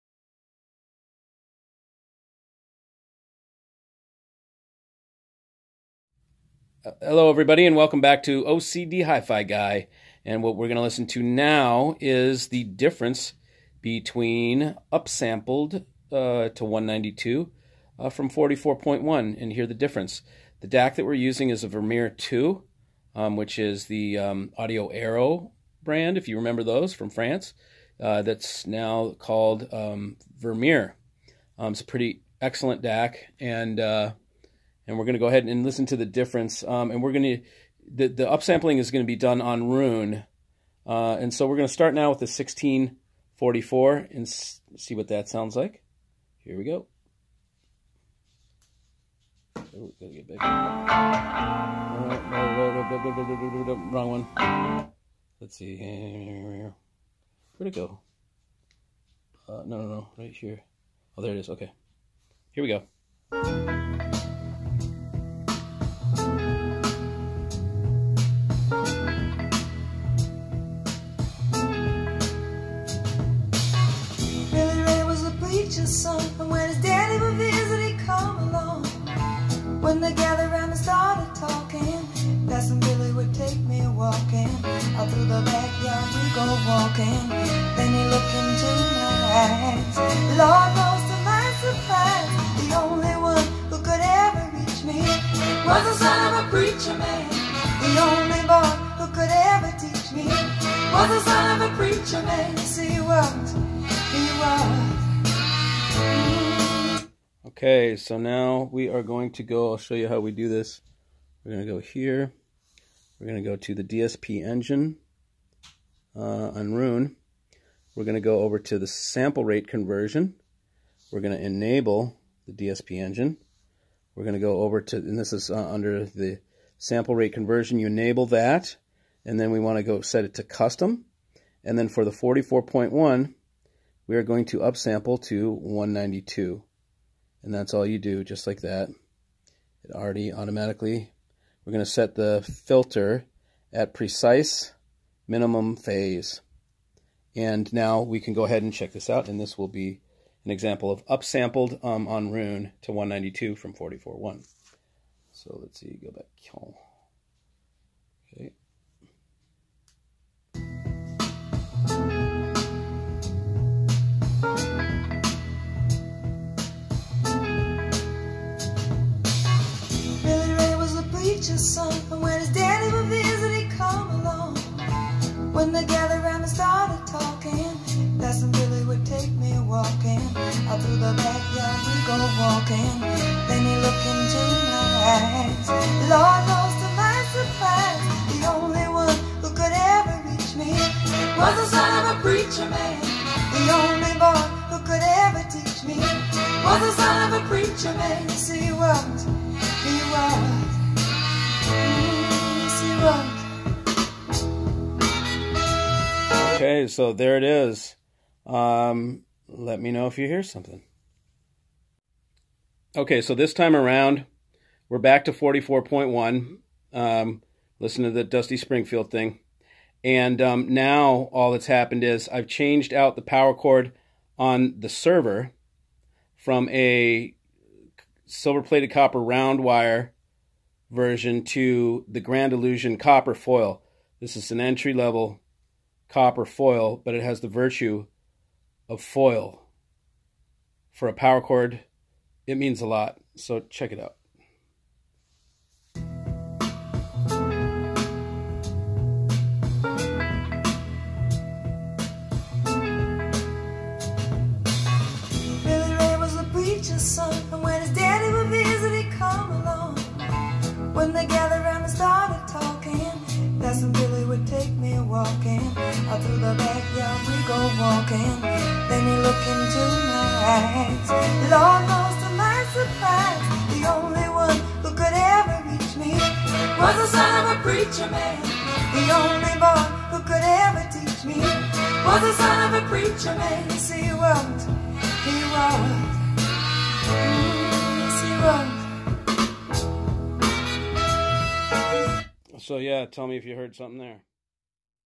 ROON upsample vs power cable
Listen as I compare the difference between a 16/44 CD rip to the same rip with DSP upsampling, and the same rip after putting an even higher grade Audiophile power cable on the music server than the silver plated copper round wire that was there